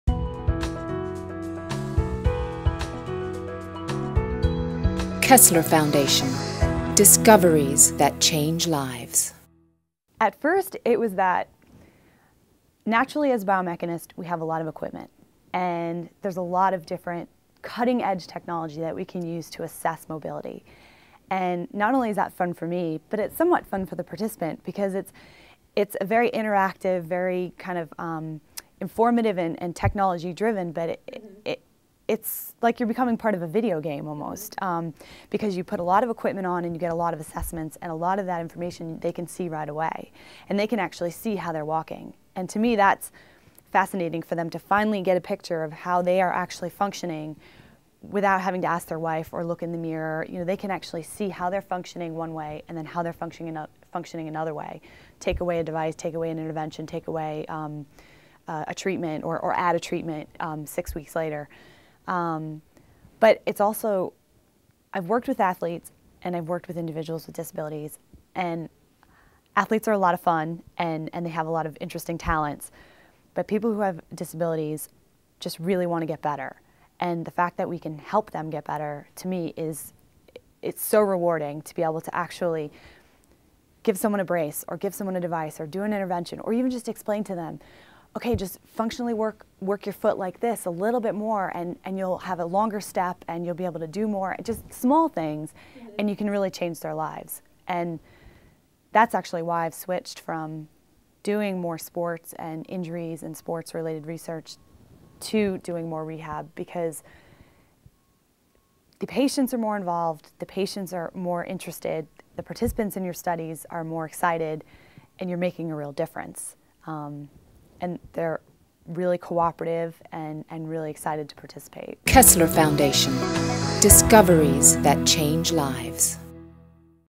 In the News with Karen Nolan, PhD #3
In the News with Karen Nolan, PhD, research scientist at Kessler Foundation Research Center. Dr. Nolan talks about what she finds most rewarding about rehabilitation research being able to help people who are so motivated to recover.